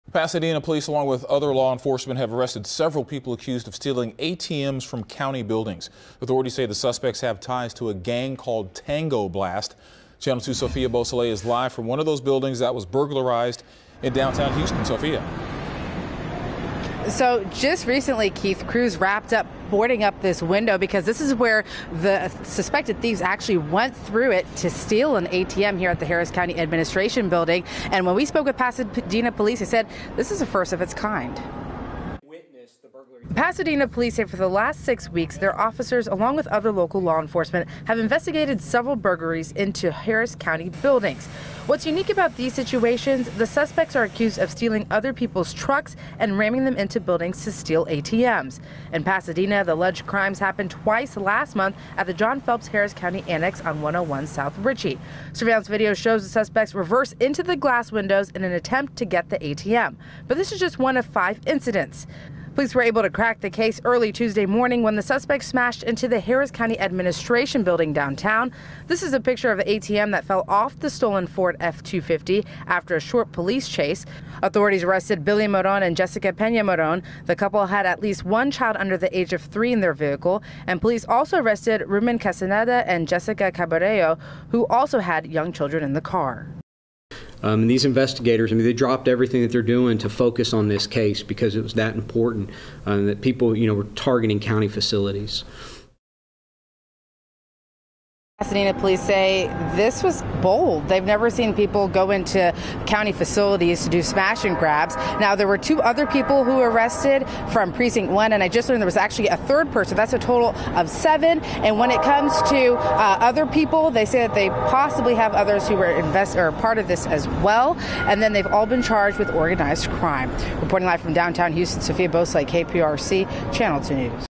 Six arrests linked to ATM thefts
Six people arrested Tuesday after an ATM theft at a Harris County building in downtown Houston are believed to be connected to a rash of similar thefts that have happened over the past couple of months, police say.